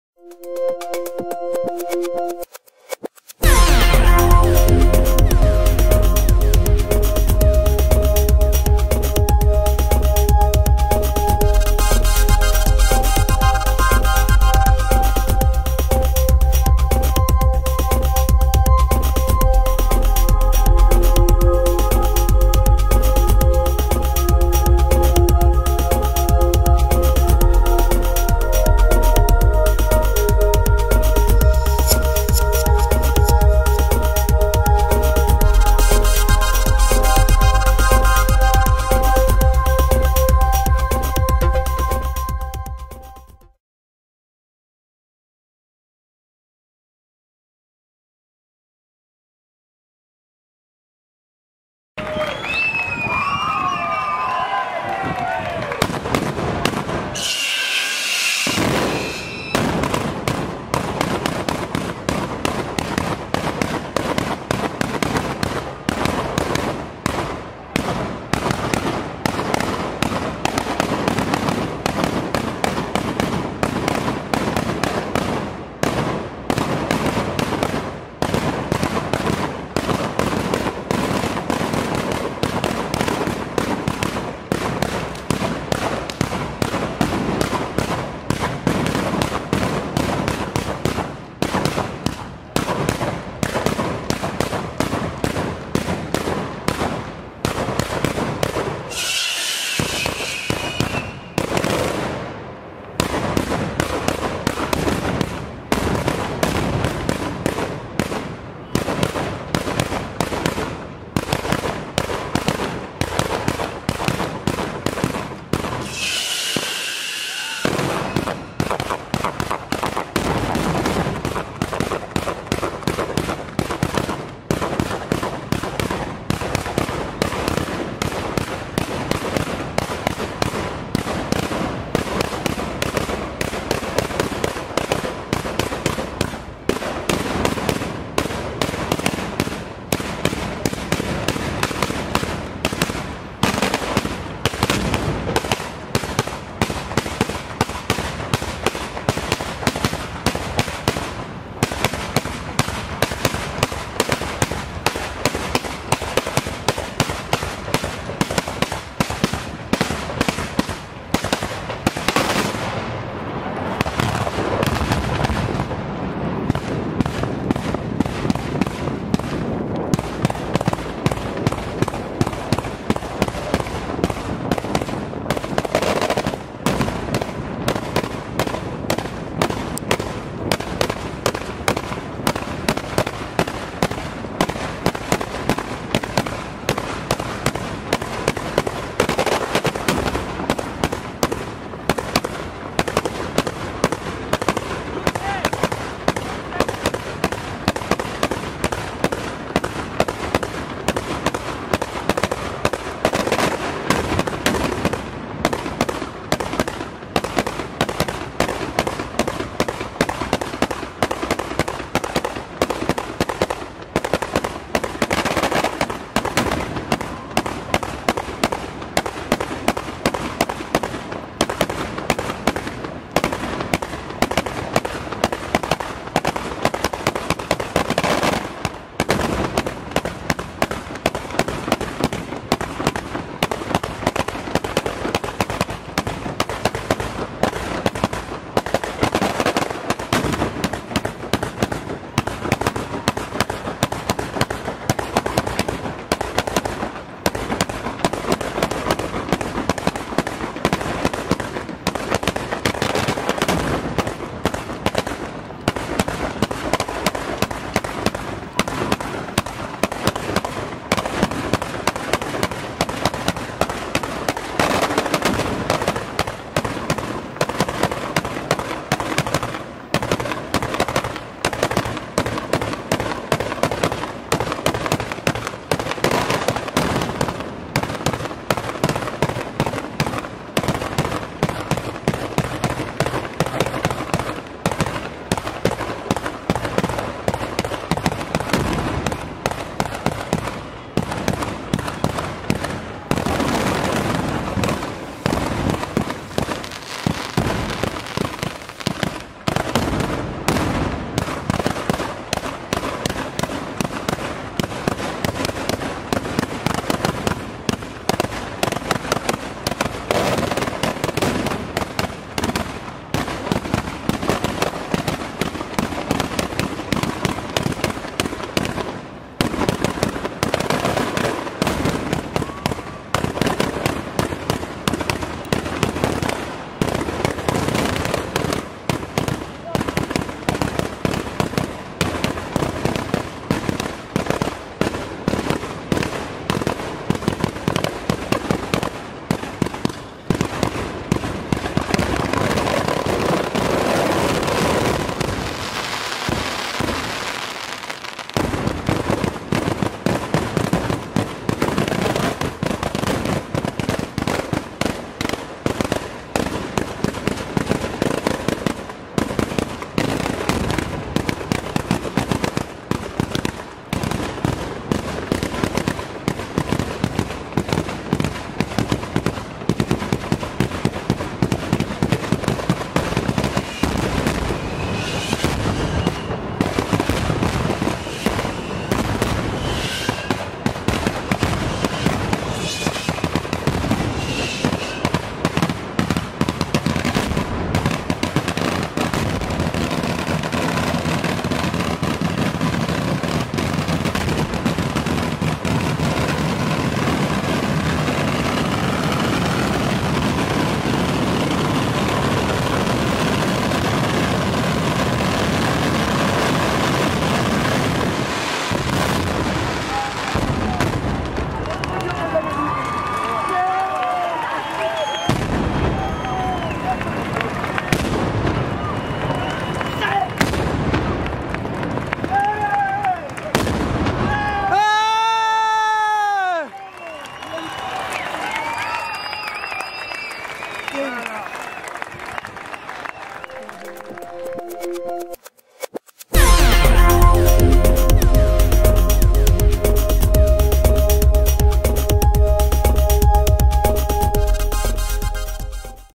RECCO (Ge) - "SAGRA del FUOCO" 2016 - Sparata Quartiere "SAN MARTINO"
Recco (Ge) 8 Settembre 2016
Festa di Nostra Signora del Suffragio - "SAGRA del FUOCO"

Sparata dei "MASCOLI" Quatiere "SAN MARTINO"